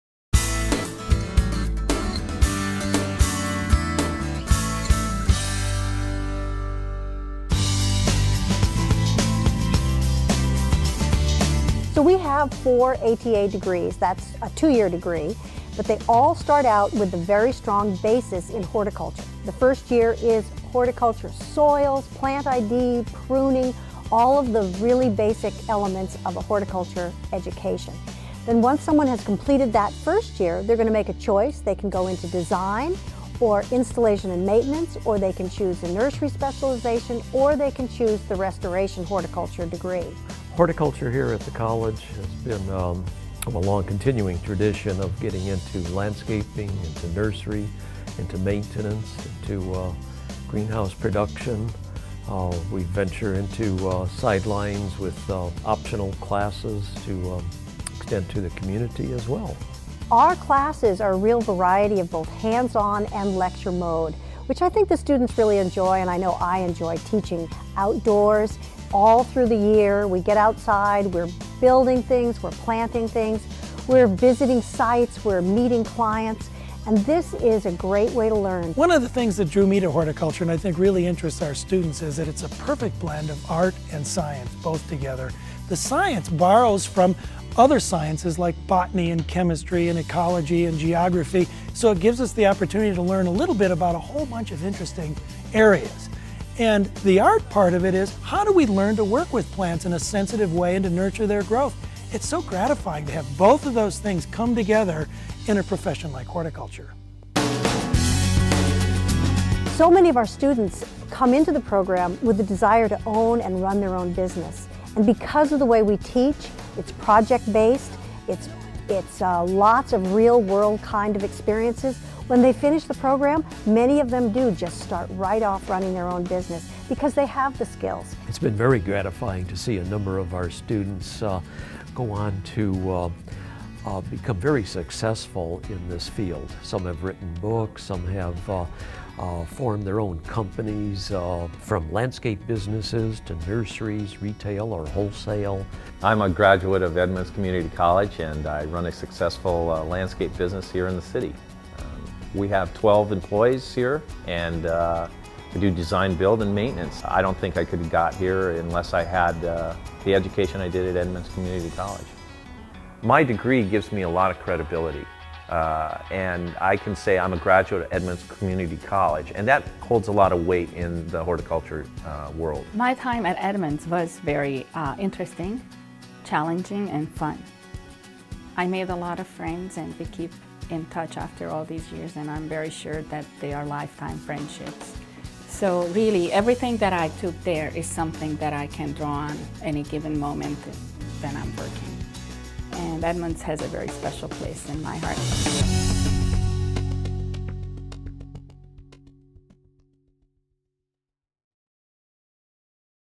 Horticulture at Edmonds Community College
A horticulture career offers creative and technical challenges that provide lasting professional and personal rewards. Job placement rates remain consistently high.